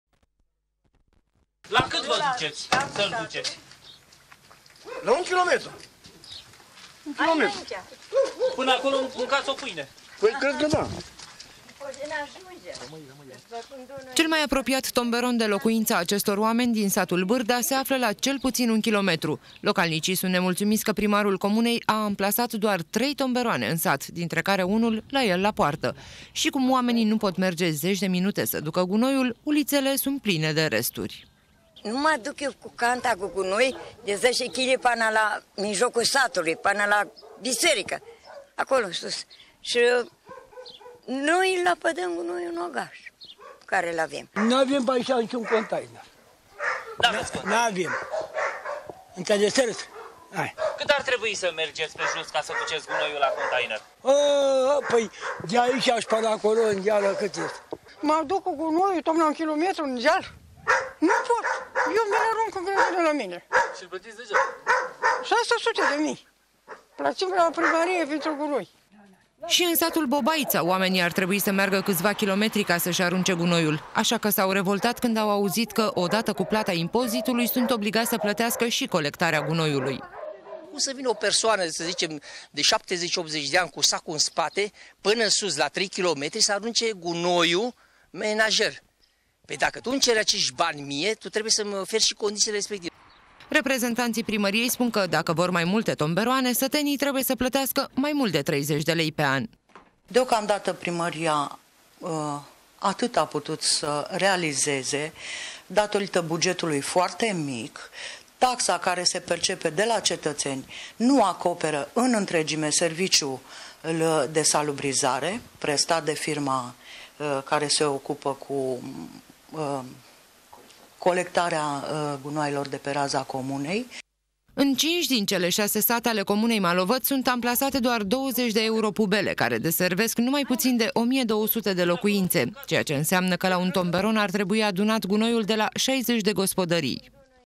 2 KM PE JOS CA SĂ DUCĂ GUNOIUL
Localnicii din comuna mehedinţeană Malovăţ sunt obligaţi să plătească taxa pentru gunoiul menajer, deşi sunt nevoiţi să meargă câţiva kilometri pentru a duce gunoiul la pubelă. Pentru că distanţa e prea mare , mulţi cetăţeni  aruncă pe uliţe deşeurile.  Autorităţile au încheiat un contract cu o firmă de salubritate, însă maşina vine doar de două ori pe lună şi goleşte numai tomberoanele. Reprezentanţii primăriei spun că oamenii primesc pentru cât plătesc.